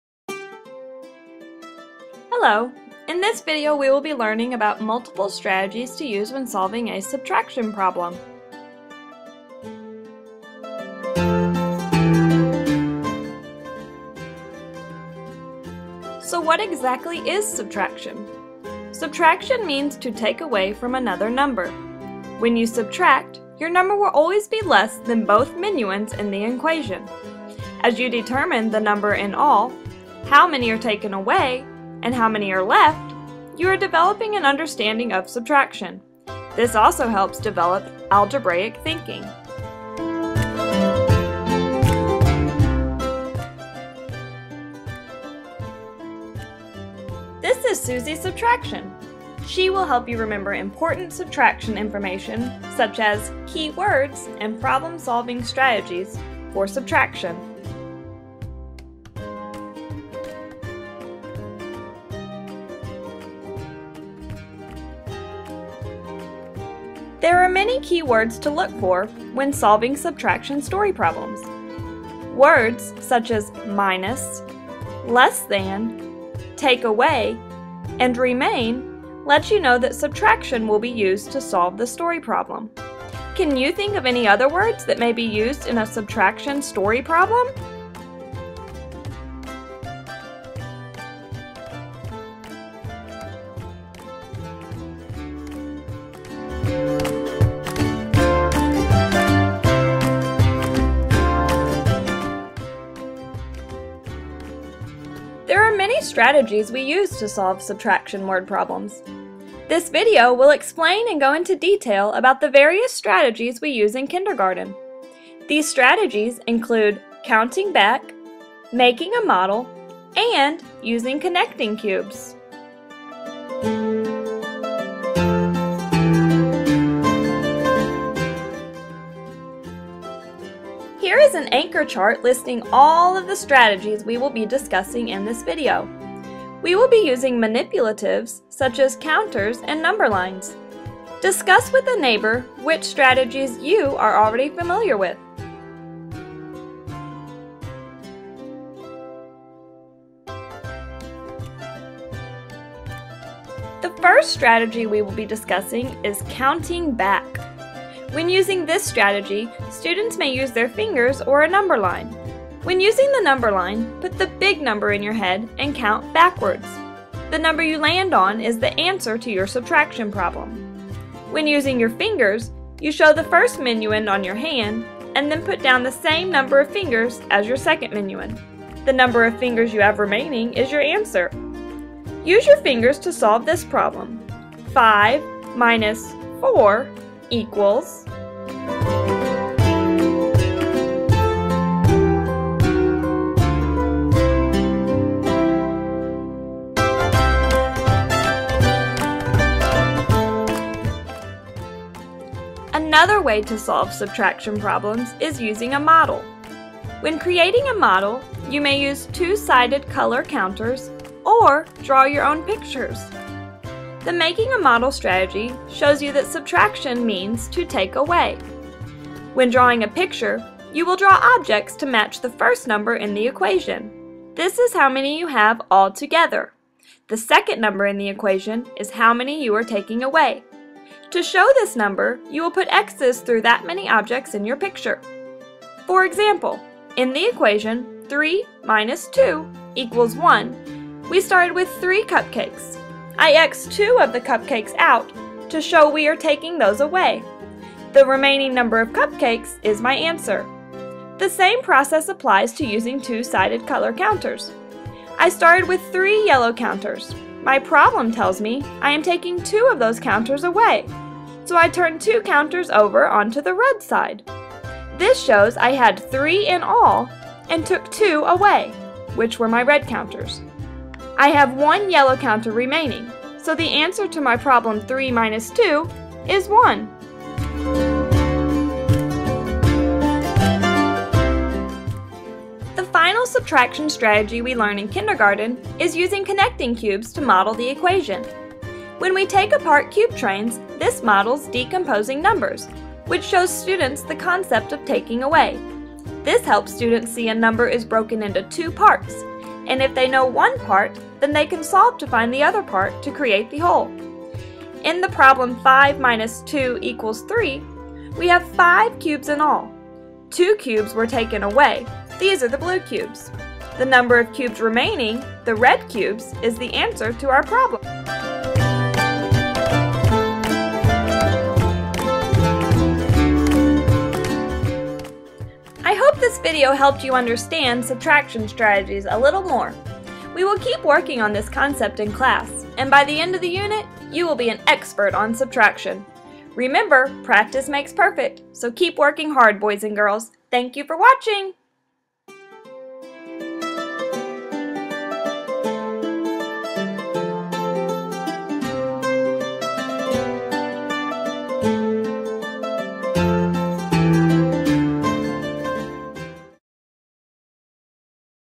Subtraction Video
A simple video about using subtraction strategies in kindergarten.